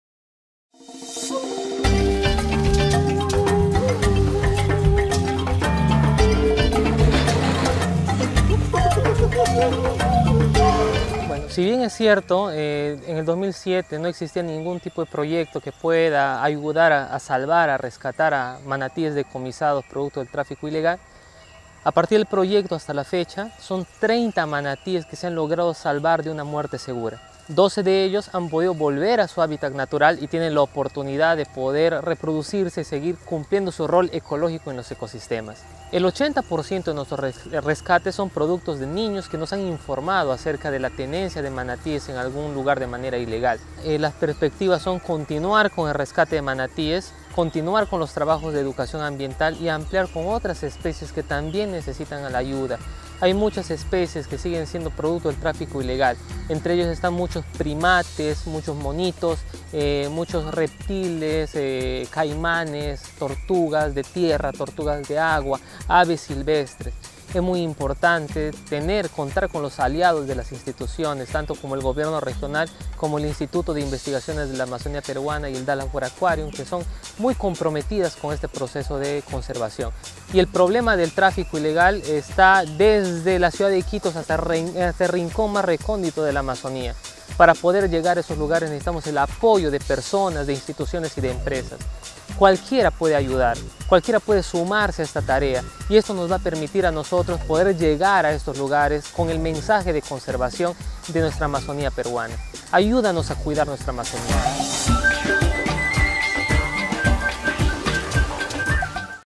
CREA vida con el Centro de Rescate Amazónico de Iquitos
Javier Velásquez, director del Centro de Rescate Amazónico (CREA), nos enseña y motiva a creer en el proyecto que salva, recupera y contribuye a una vida integral y plena a varias especies de la fauna amazónica.

Ayuda y difunde este mensaje de conservación de la Amazonía. CREA Vida